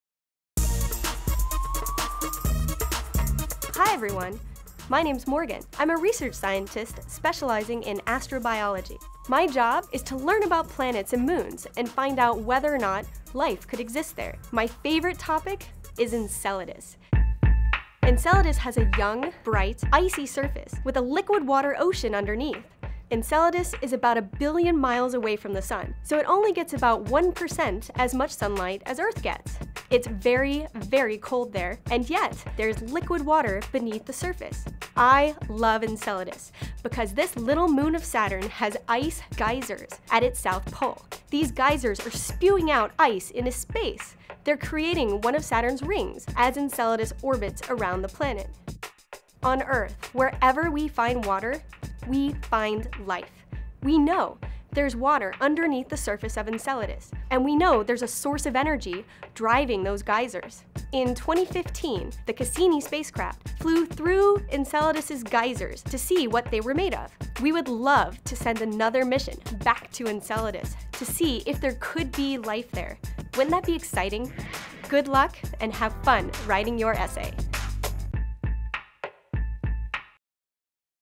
Scientist for a Day Option 1: Enceladus
Continuing in the tradition started by the Cassini mission to Saturn, Scientist for a Day challenges students in grades 5-12 to think like NASA scientists. Examine real spacecraft images of Saturn’s moon Enceladus, Saturn’s moon Titan, and Jupiter’s moon Europa.